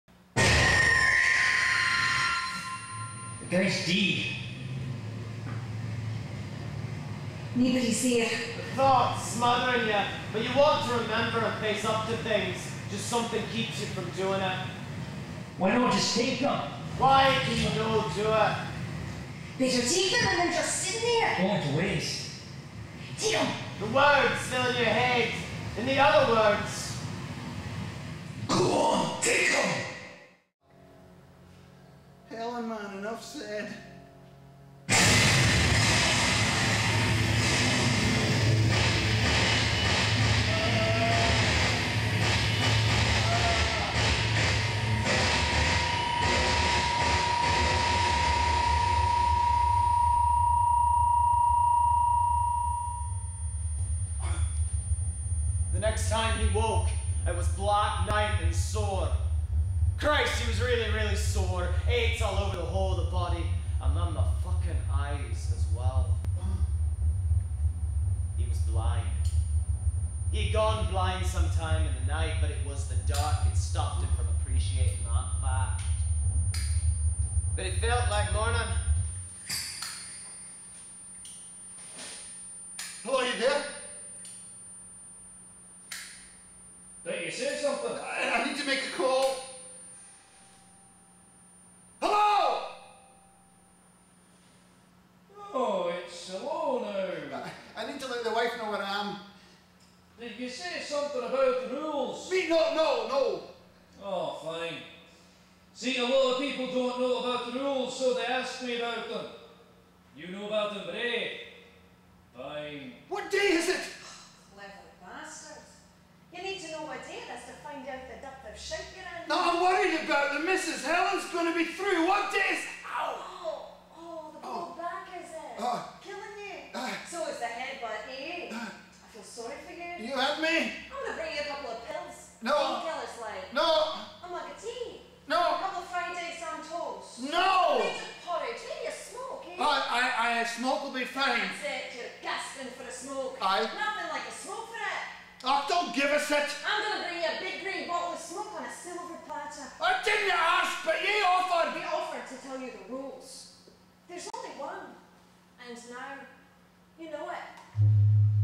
How Late It Was How Late
Line Dash's video design included 3 cameras, 3 screens, 2 DVD players, 2 switchers and a theater perimeter painted completely chroma key green.  rudemechs.com